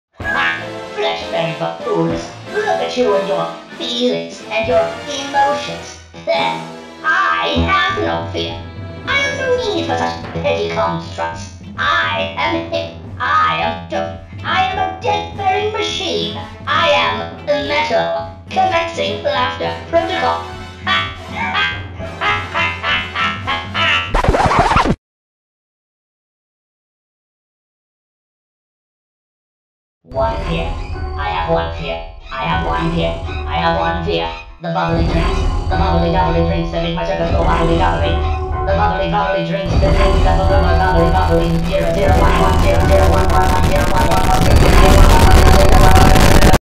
MechaGodzilla's One Fear | Monsterverse | Comic Dub
Actually found this on my drive a while back and decided that it was too perfect to change so here is another comic featuring MechaGodzilla! X3 Hope you all enjoy! Roar With Excellence!

(CAST):
MechaGodzilla - Levi Keiser

(CREDITS):
Art(C) - @ruubesz
Godzilla(C) - Toho Co. Ltd, Warner Bros. Pictures, and Legendary Entertainment